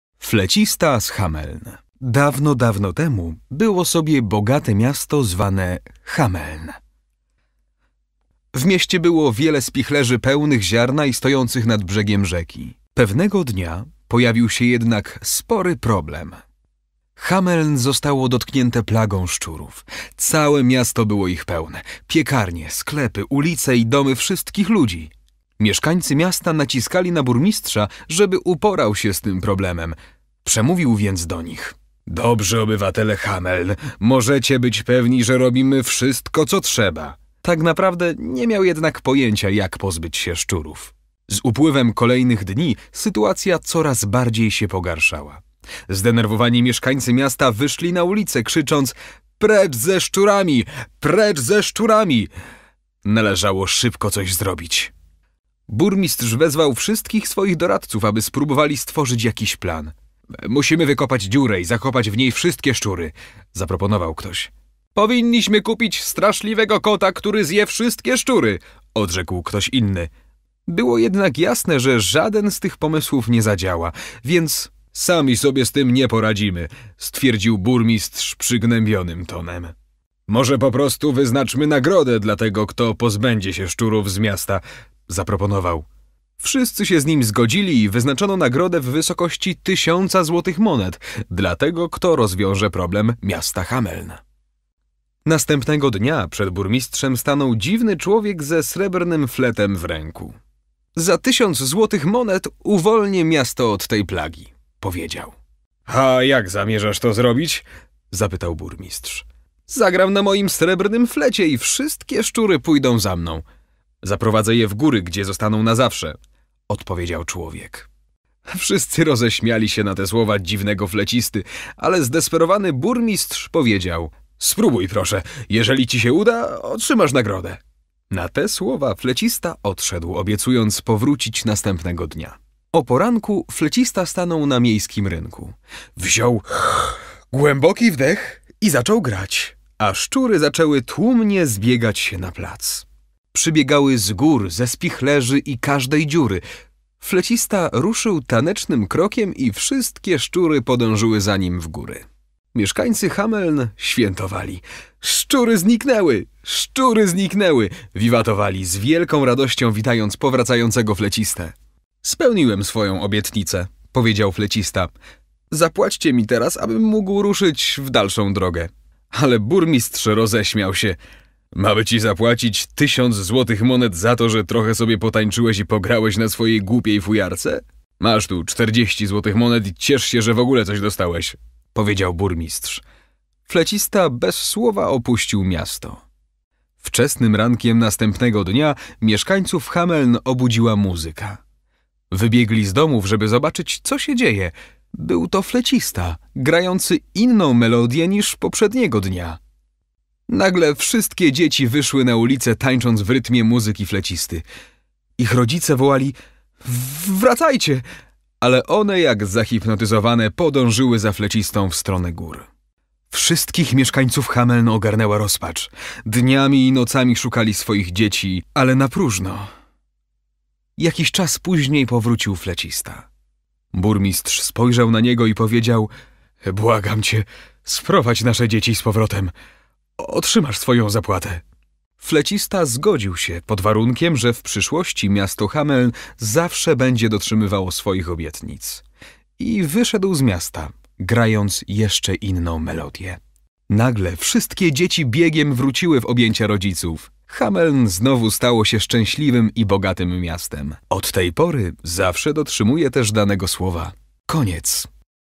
FLECISTA Z HAMELN
Miasto Hameln zostaje dotknięte plagą szczurów. Nagle tajemniczy flecista proponuje rozwiązanie problemu...